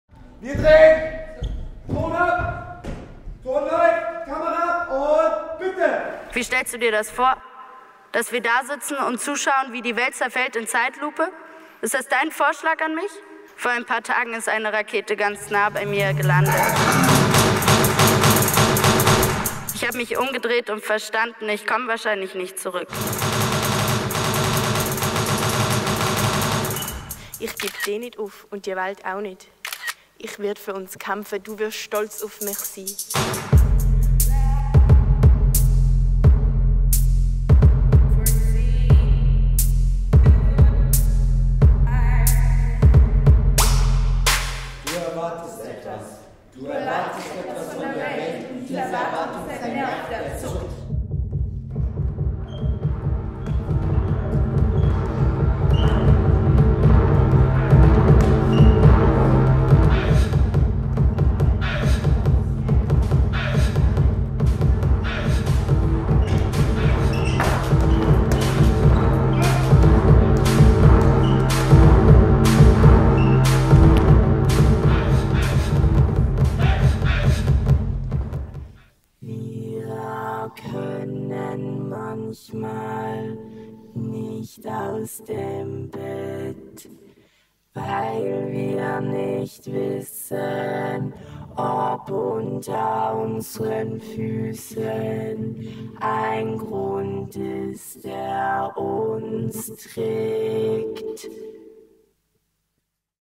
Zucken (Trailer)
Von Sasha Marianna Salzmann | Regie Sebastian Nübling

Da ist eine junge Frau, die im Chat die große Liebe findet, sich ihr anvertraut und bereit ist nach Syrien auszureisen. Als ihr Plan scheitert, packt sie ihren Rucksack voll mit Messern und geht zum nächsten Bahnhof. Oder der junge Mann, von dem verlangt wird sich zu bekennen: Bist du Russe oder bist du Ukrainer? Er flieht zu seinem Freund, vor dem ihn die Eltern gewarnt haben.
»Du erwartest etwas, du erwartest etwas von der Welt und diese Erwartung ist ein Nerv, der zuckt.«

Sasha Marianna Salzmanns neuer Theatertext Zucken erzählt von jungen Menschen, die eine zunehmende Distanz zwischen sich und den gewöhnlichen Verunsicherungen des Alltags empfinden: politische Zweifel, sexuelle Überraschungen und die Unmöglichkeit, den großen, ehrlichen Fragen mit Gleichgültigkeit zu begegnen. Warum begeben sich Menschen, deren Leben gerade erst so richtig beginnt, auf die Suche nach radikalen Alternativen zu dem, was der gesellschaftliche Konsens für sie vorsieht?

Eine Produktion des jungen theater basel und des Maxim Gorki Theater Berlin. Die Arbeit von Sasha Marianna Salzmann wurde gefördert durch die Heinz und Heide Dürr Stiftung. Die Aufführungen in Berlin werden durch prohelvetia unterstützt.

Premiere: 17. März 2017

TEAM

REGIE Sebastian Nübling
AUSSTATTUNG Ursula Leuenberger
SOUND Lukas Stäuble
DRAMATURGIE Ludwig Haugk, Uwe Heinrich

BESETZUNG

Martha Benedict
Yusuf Çelik
Doğan Çoban
Elif Carci
Timo Muttenzer
Helena Simon
Cara Stauffenegger